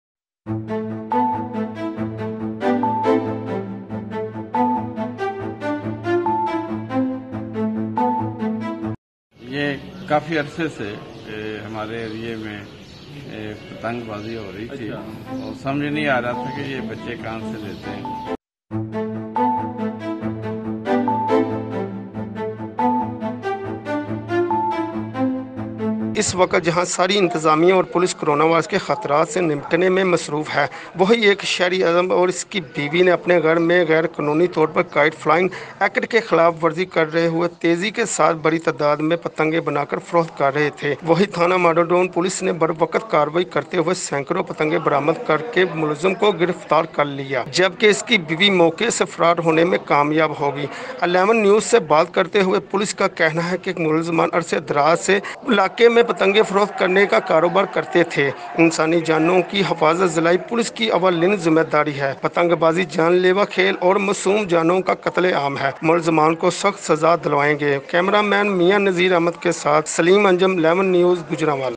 گجرانوالہvماڈل ٹائون تھانہ نے پرپتنگیں بنا کر فروخت کرنے والے ملزم کو گرفتار کرلیا گیا
گجرانوالہماڈل ٹائون تھانہ نے پرپتنگیں بنا کر فروخت کرنے والے ملزم کو گرفتار کرلیا گیا